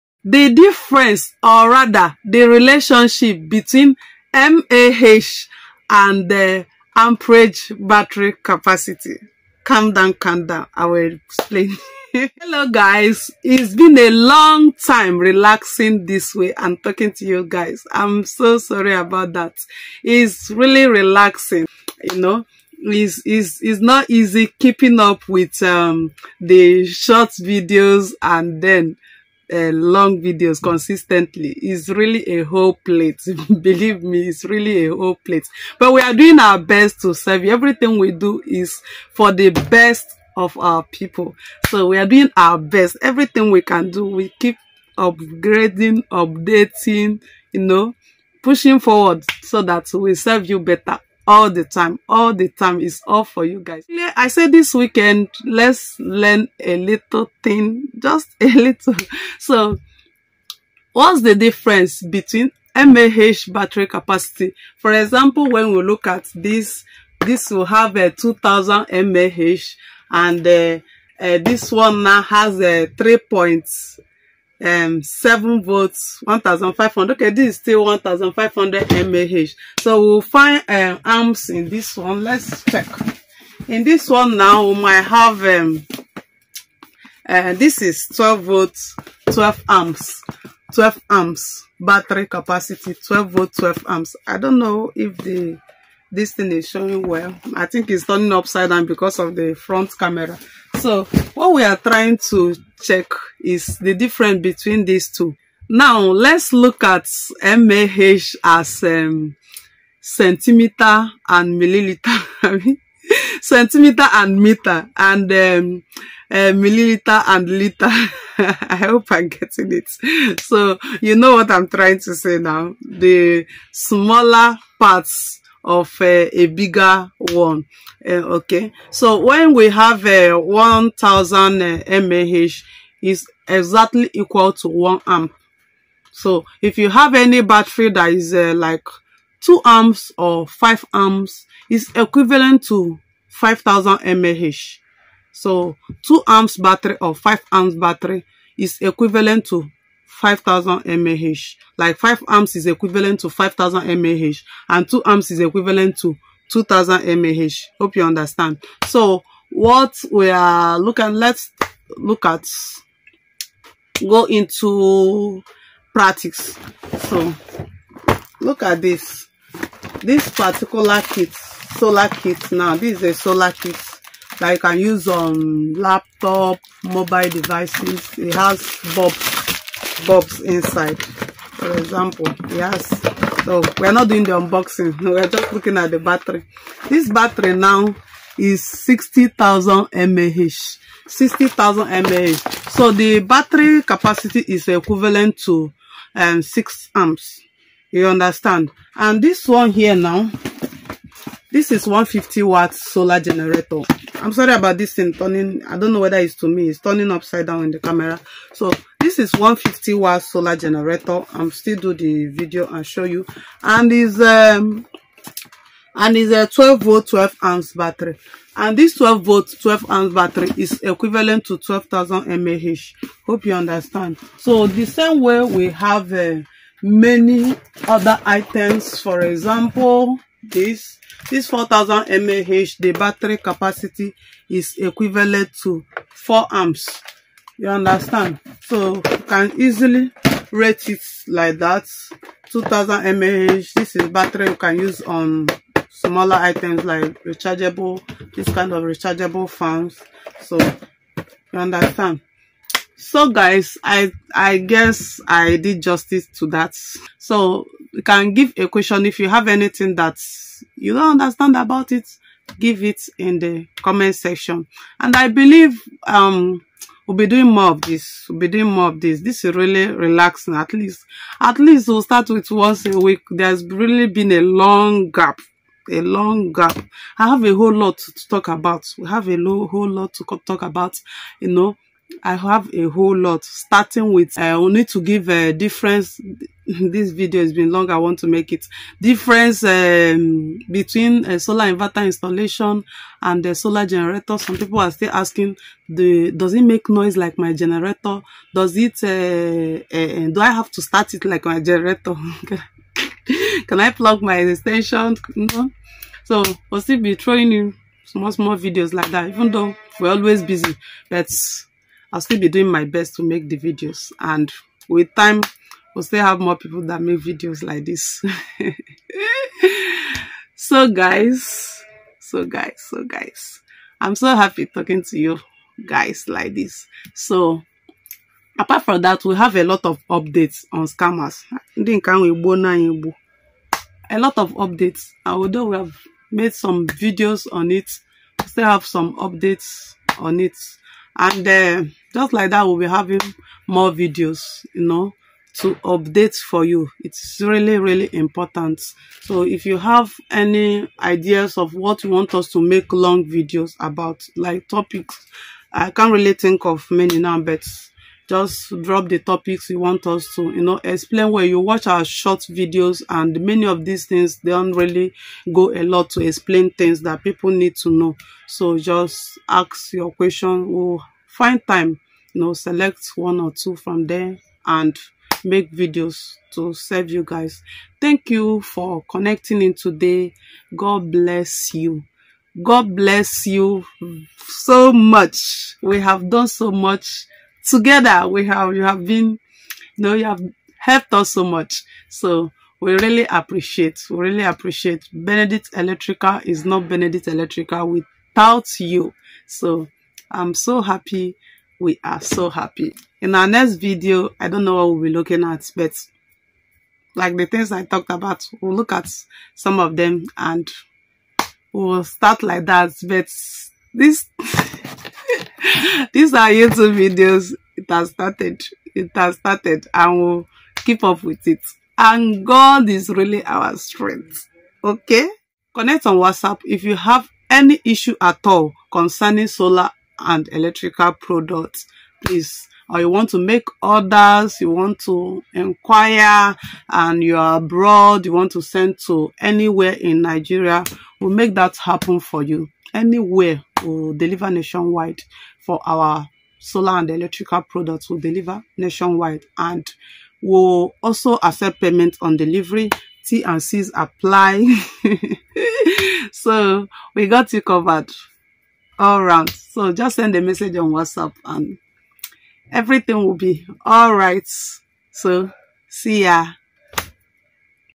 How to rate Mah in Amperage Batteries - Difference and Relationship between the two
Simple tips on how to differentiate two different battery capacities and their relationship as well.
Amperage and Mah batteries are mostly seen on solar rechargeable AC/DC home appliances and even separately built batteries...